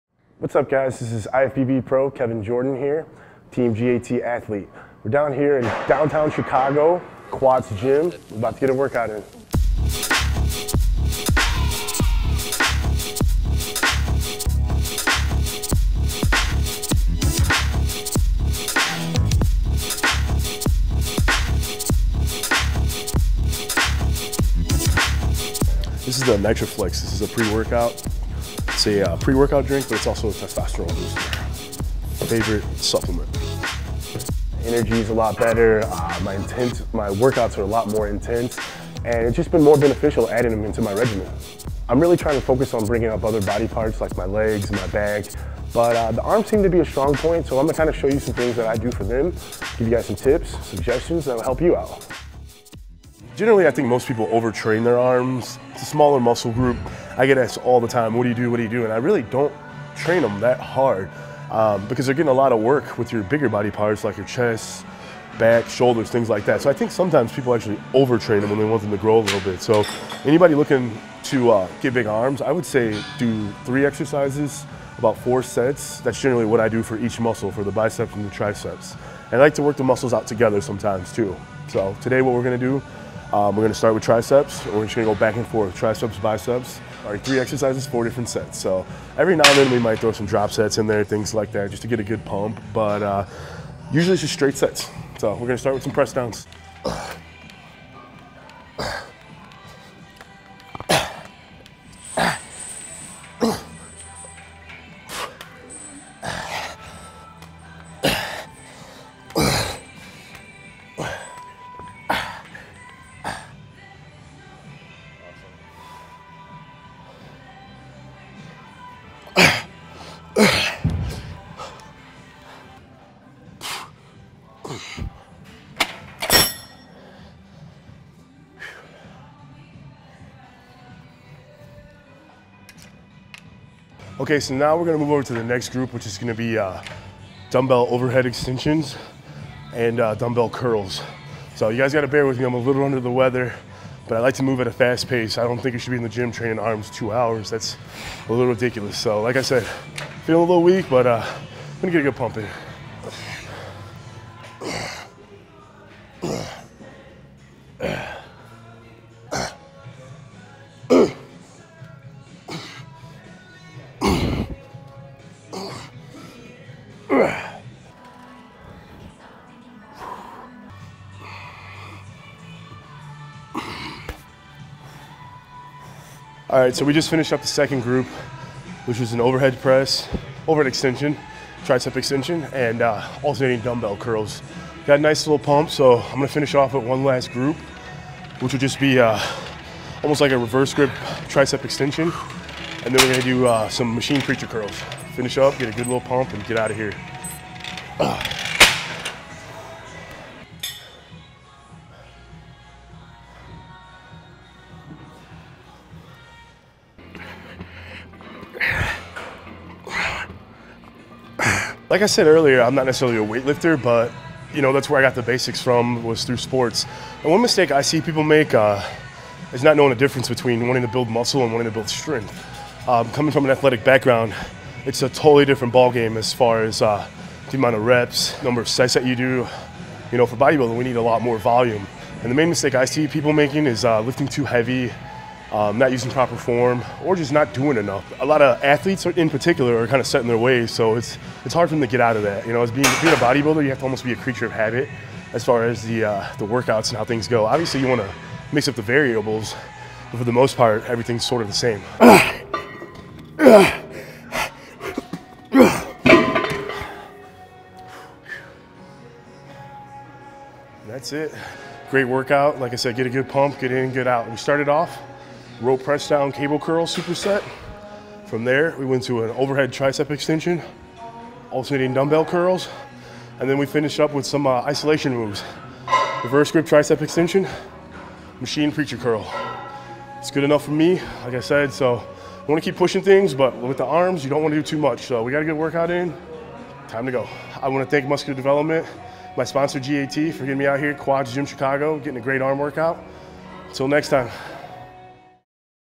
GAT Athlete Kevin Jordan | Arm Training
IFBB pro bodybuilder Kevin Jordan is fresh off his rookie season. He placed fourth in his pro debut, and never placed out of the top ten all season long.

In 2016 he’ll be focusing on adding some size to his legs and back – his self-proclaimed weaker parts. When we caught up with him at Quad’s Gym in downtown Chicago, he shared some tips and pointers for arms, which are no doubt a stronger body part for Kevin.

Kevin goes over his complete workout and gives his thoughts on training arms. He also shares which GAT product he uses before he works out to help him push through the workout and boost his testosterone.

Kevin Jordan’s Arm Workout:
Superset: Rope Press down/Cable Curls
Overhead Triceps Extension
Alternating Dumbbell Curls
Reverse Grip Triceps Extensions
Machine Preacher Curls